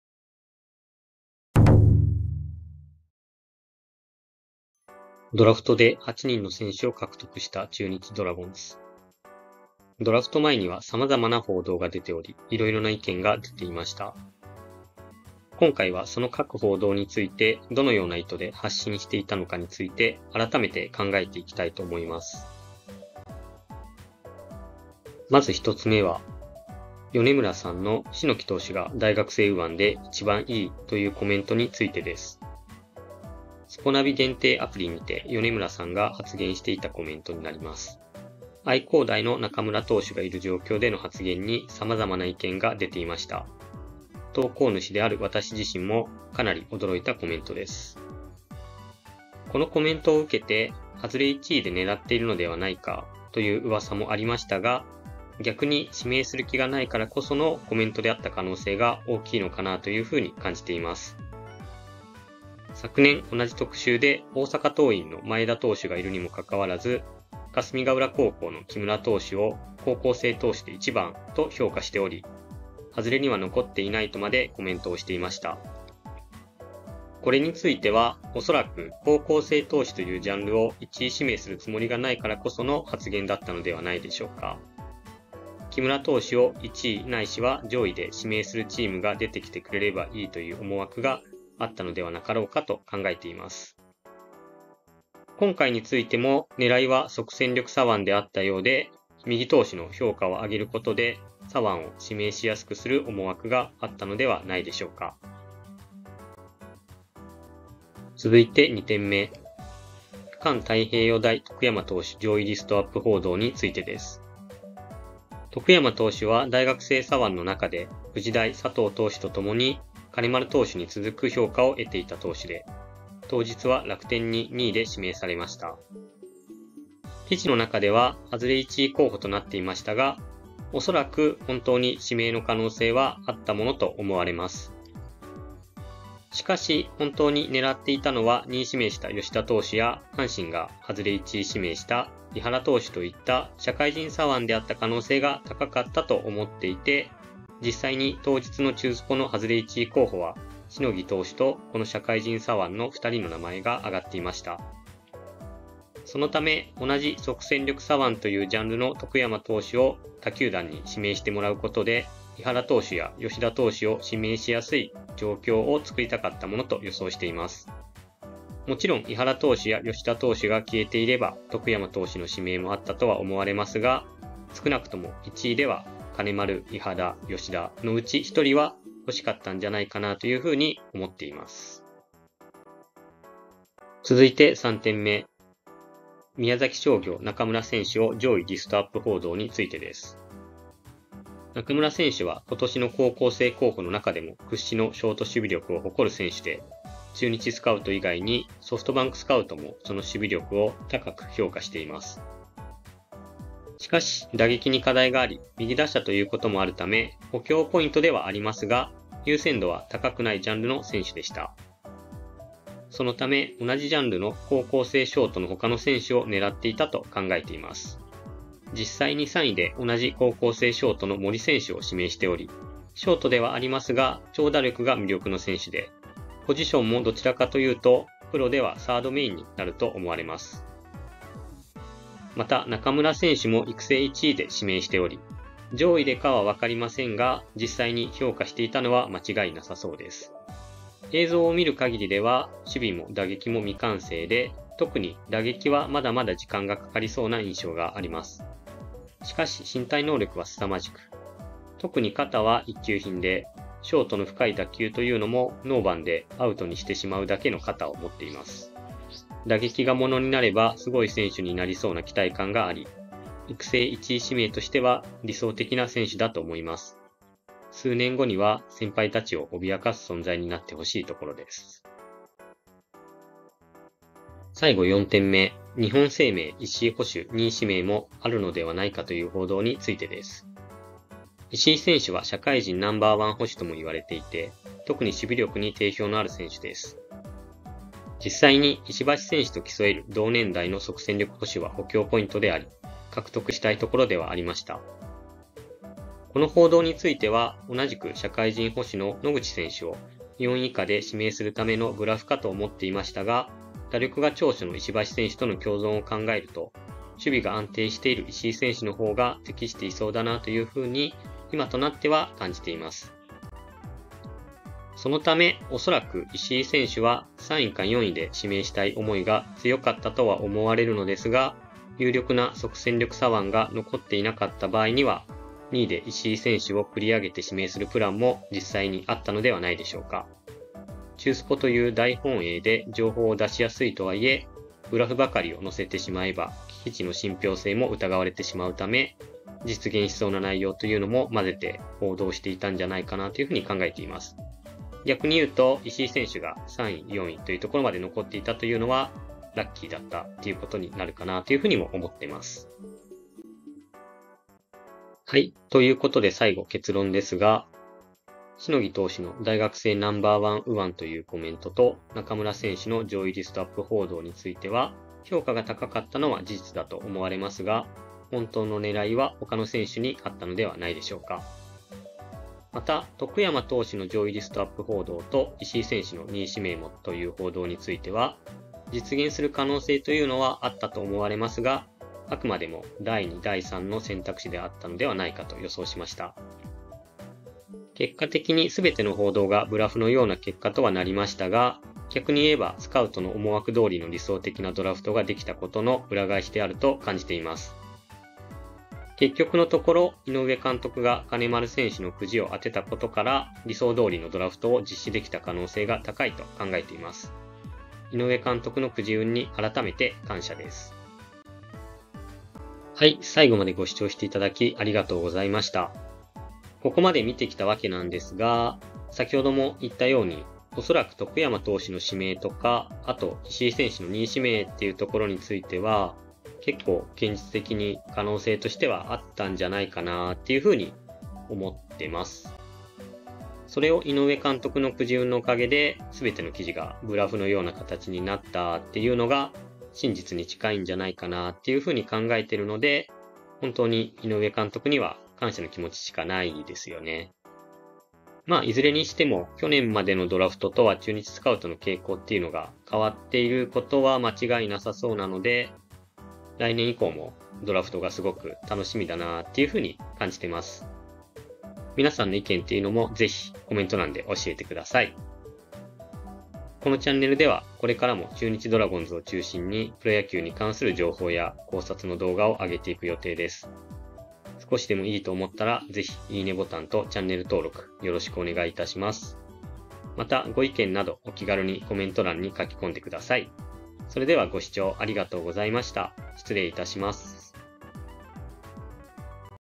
中日ドラゴンズ 神ドラフト後に振り返る！あのドラフト報道は何だったのか？
このチャンネルでは中日ドラゴンズを中心にプロ野球に関する話題や情報についての考察動画を上げていきます。